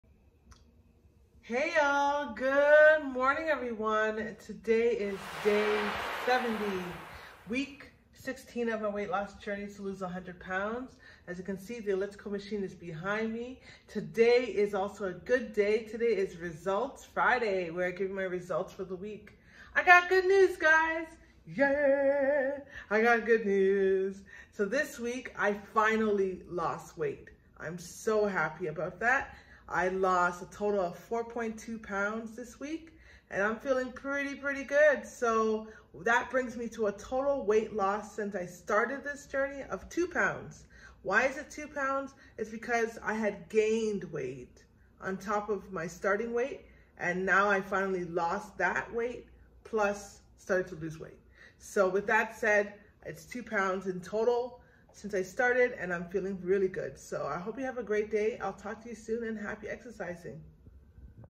Day 70 - Results Friday
Good afternoon everybody! Today is Day70 of Week16 of my Weightlossjourney Weightlosstransformation to lose 💯 lbs. Today is also ResultsFriday where I share my results for the week. Enjoy and happy exercising!